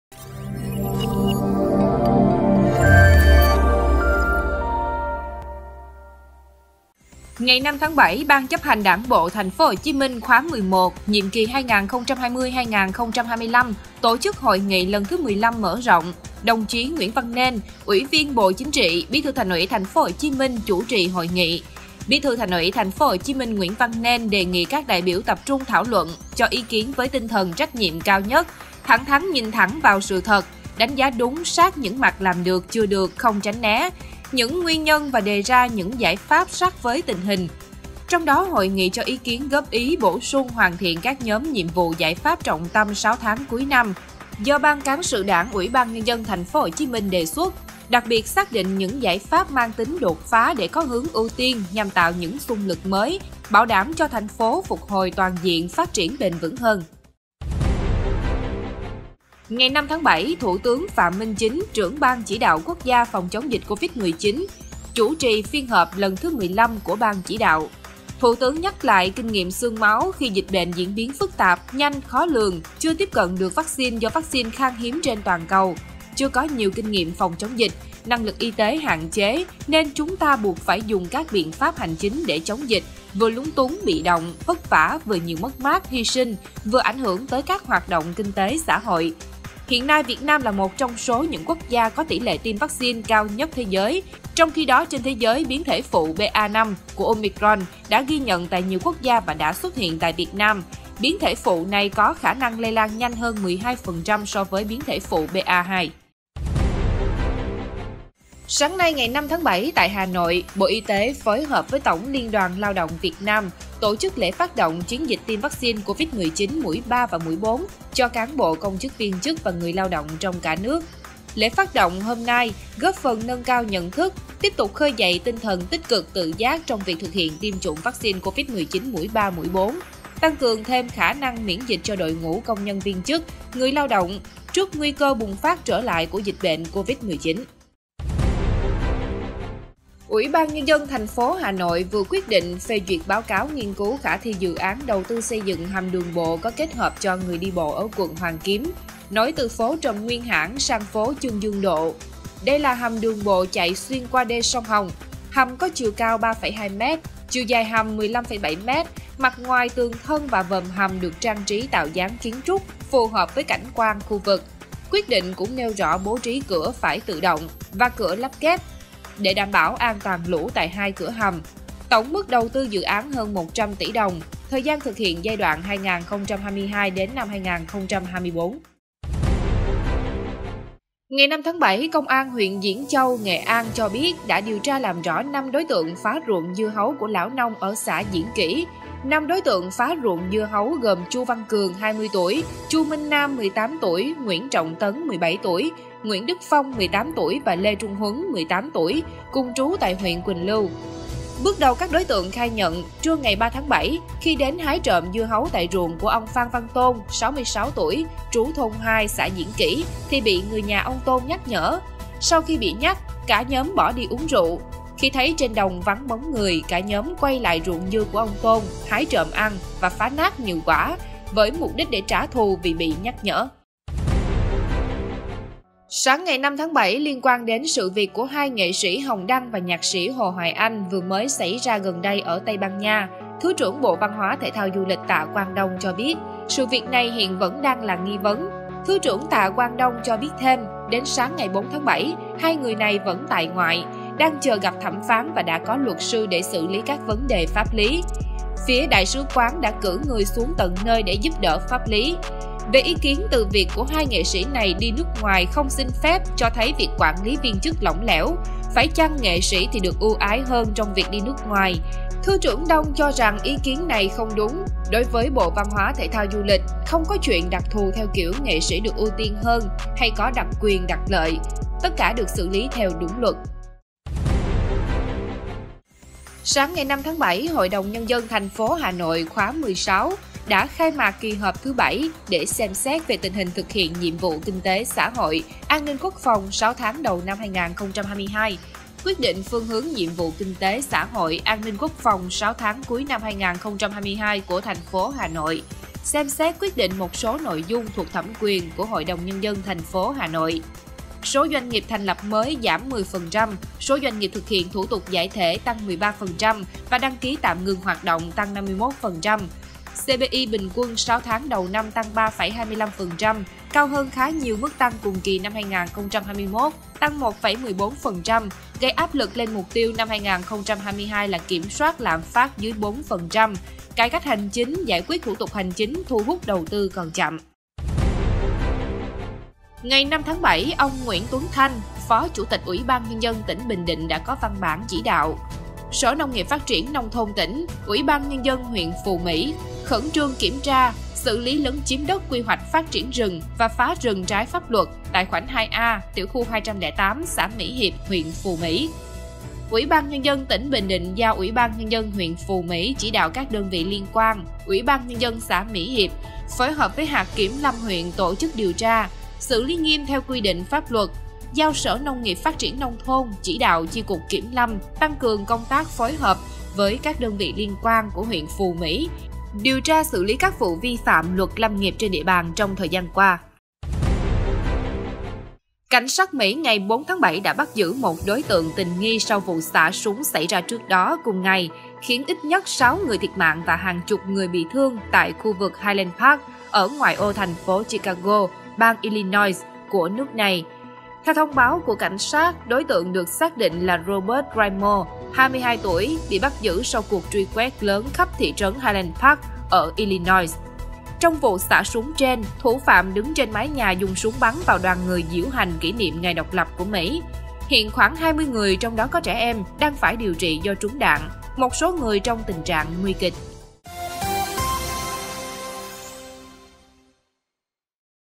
2 nghệ sĩ Hồng Đăng và Hồ Hoài Anh vẫn tại ngoại và chờ gặp thẩm phán ở Tây Ban Nha
Bí thư Thành ủy TPHCM: Tìm giải pháp đột phá tạo xung lực mới cho thành phố phục hồi toàn diện; Thủ tướng yêu cầu không được quên “kinh nghiệm xương máu” khi chưa có vaccine Covid-19; Bắt đầu chiến dịch tiêm vaccine Covid-19 mũi 3 và 4 bảo vệ cán bộ, công chức, người lao động; Nghệ An: Phá nát ruộng dưa hấu của lão nông vì hái trộm bị nhắc nhở; Lãnh đạo Bộ VH-TT-DL: 2 nghệ sĩ Hồng Đăng và Hồ Hoài Anh vẫn tại ngoại và chờ gặp thẩm phán… là những nội dung đáng chú ý có trong Điểm tin SGGP Online ngày 5-7-2022.